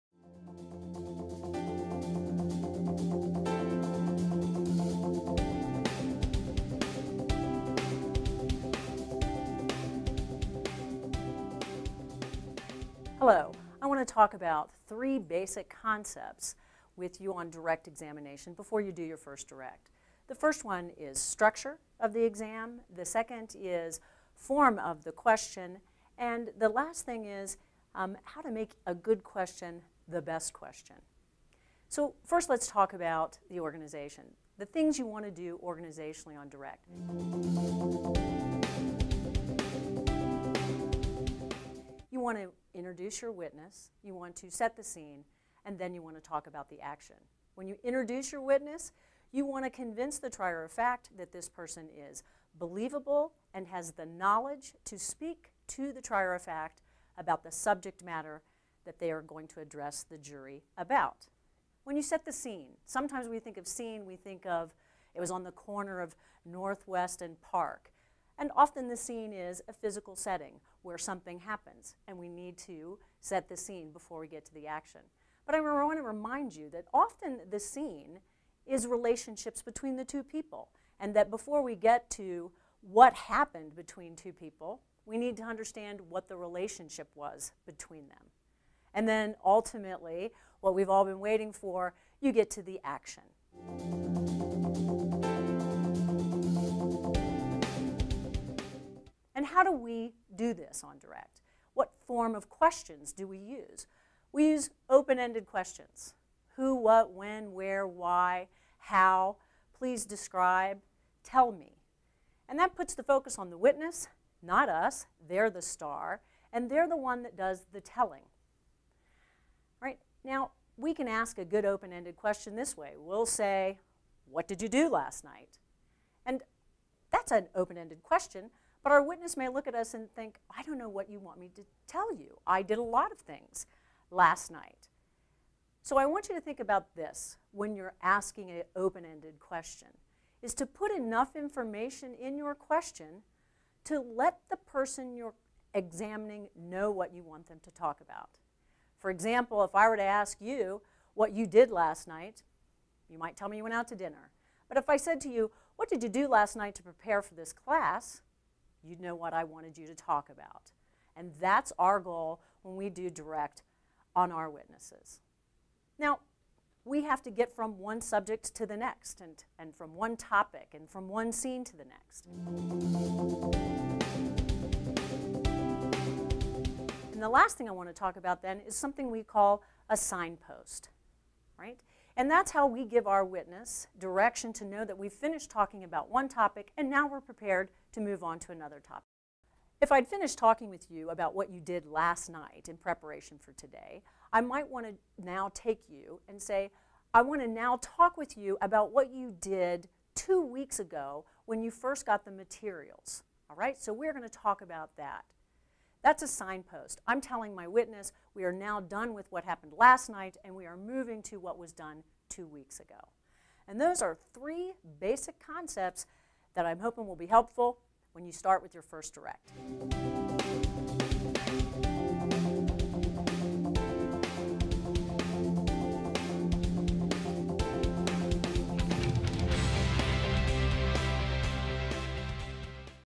NITA Faculty Mary Jo Barr - Key Elements of Direct Examination
Mary Jo Barr explains 3 key elements that will help when doing your first Direct Examination.